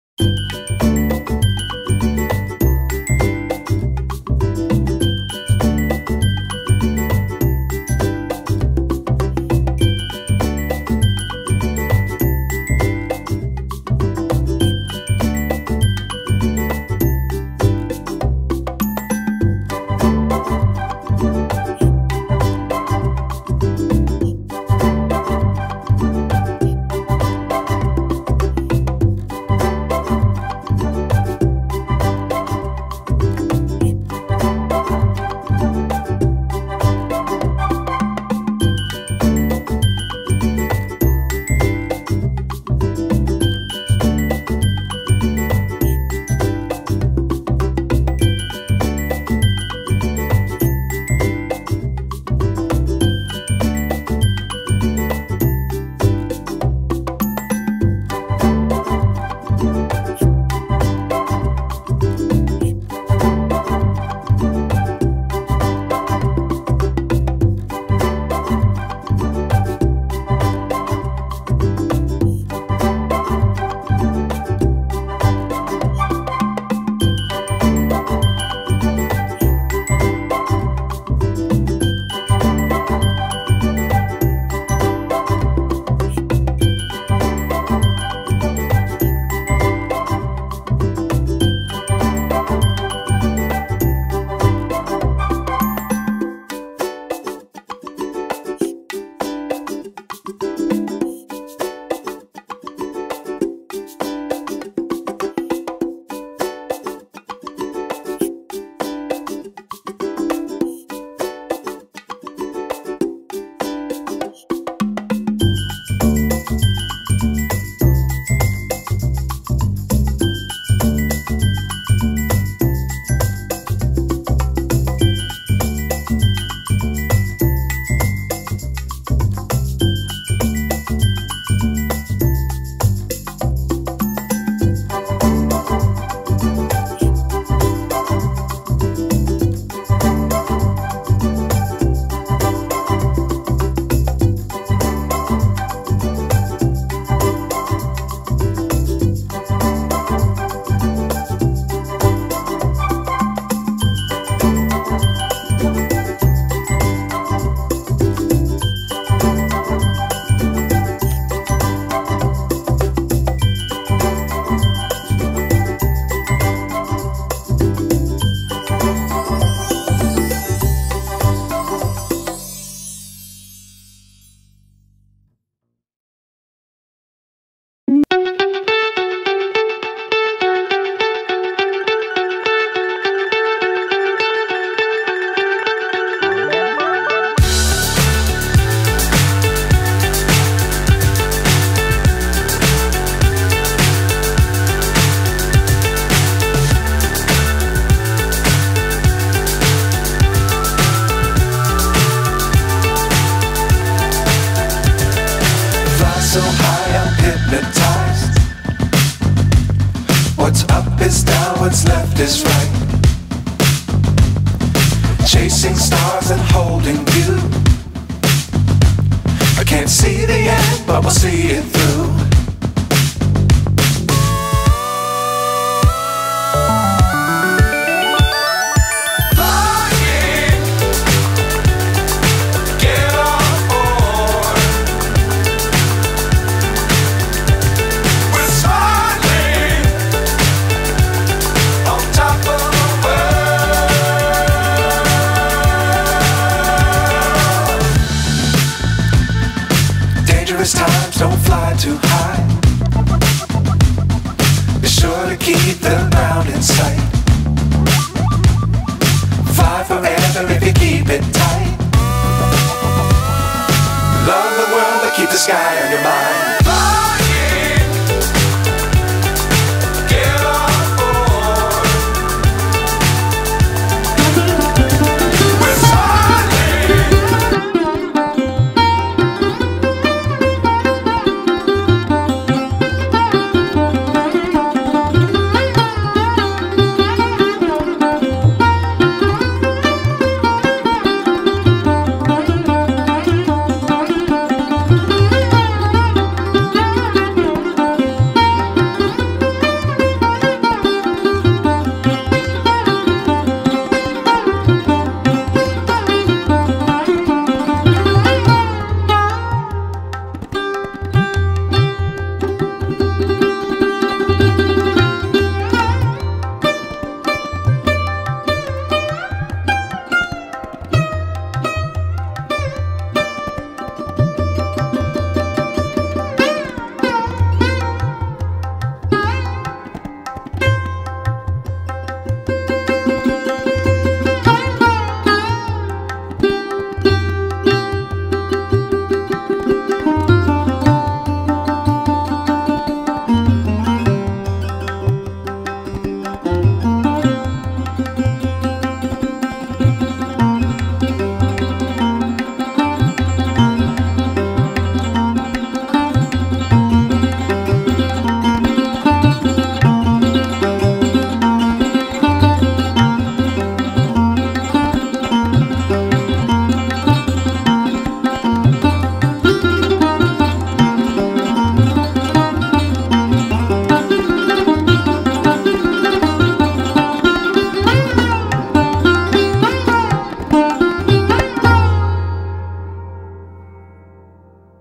Amazing Cardboard craft😱😍 | Home Decor | clay peacock design
You Can Make this Fresh Festive Home Decor Ideas at home

Material used
Waste Cardboard
Mouldit clay
acraylic colors
fevicol
mirrors